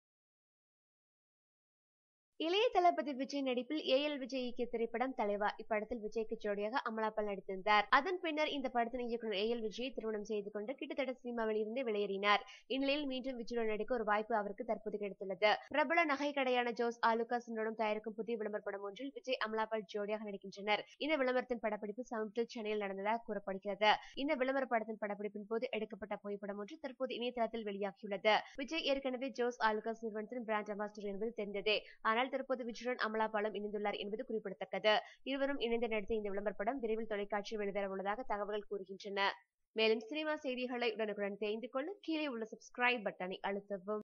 Vijay and Amala Paul to team up again
Vijay and Amala Paul to team up again for Jos Alukkas Ad